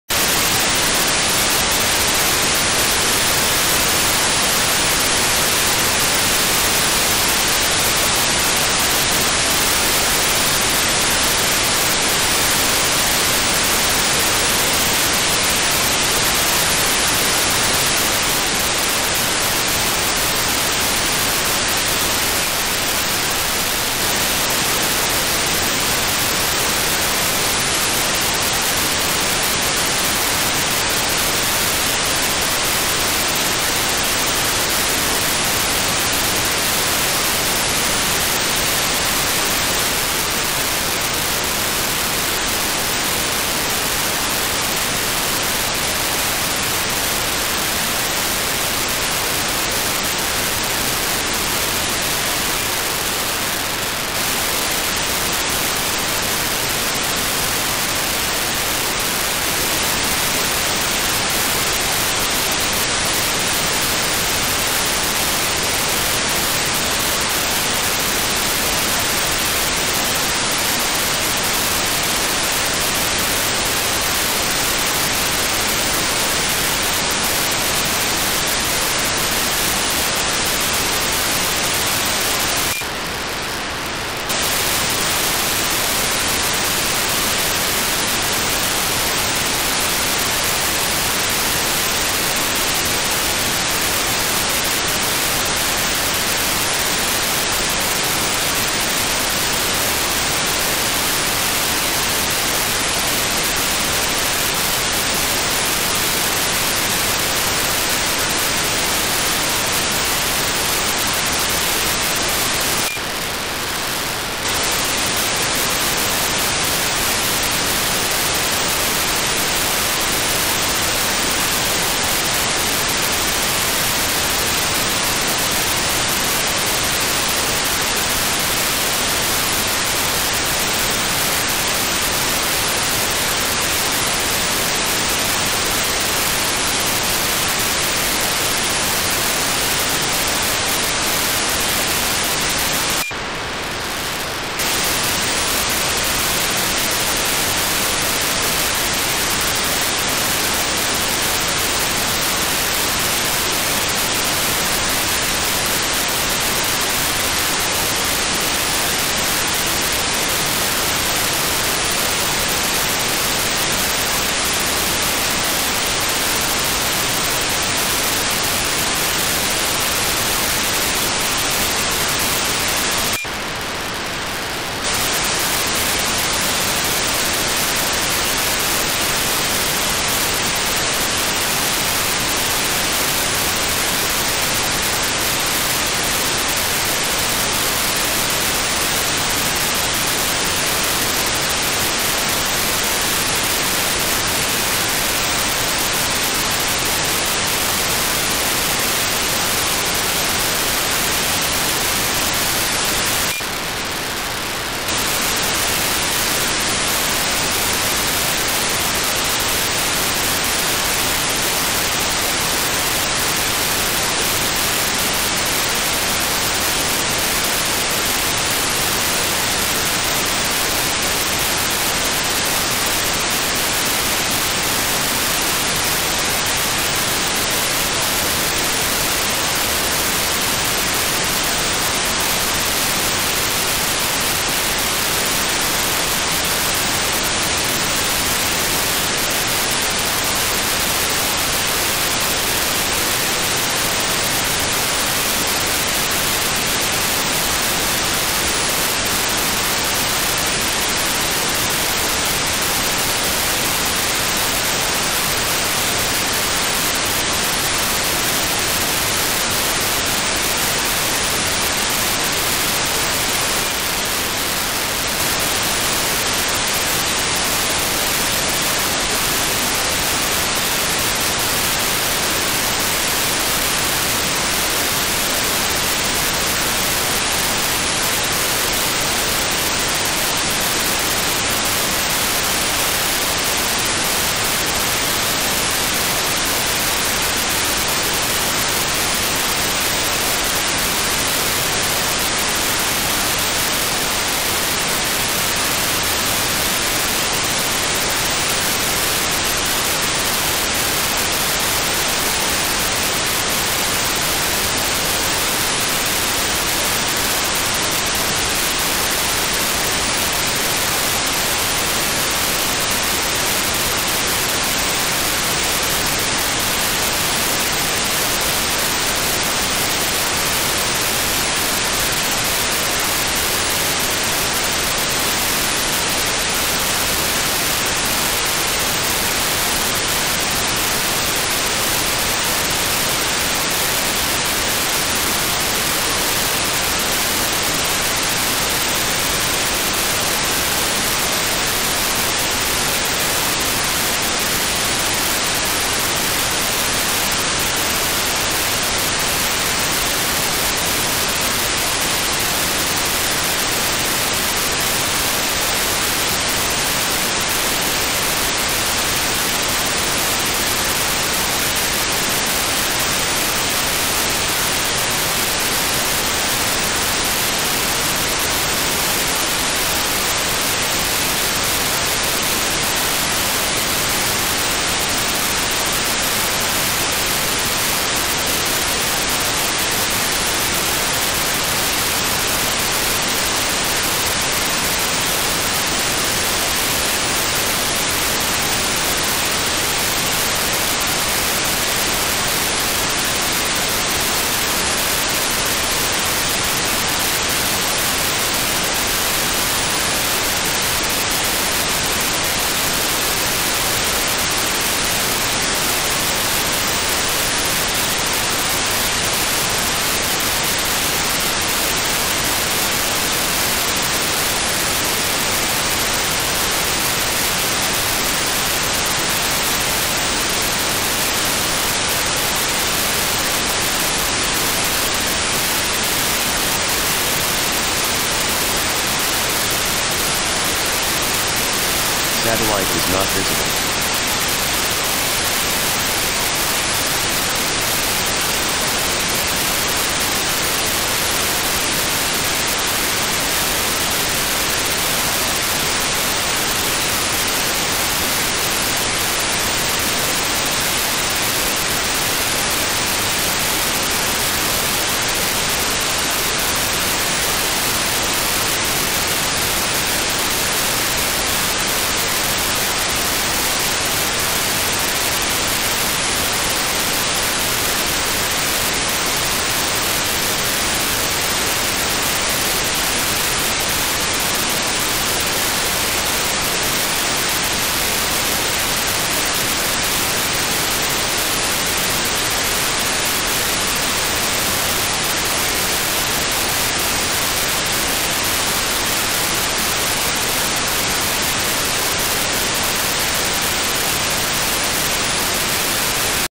2019-02-02: LUME-1: GMSK4k8 (FM) @ 437.0605 MHz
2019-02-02: GMSK4k8 (FM) signal from LUME-1 satellite at 437.0605 MHz with RTL SDR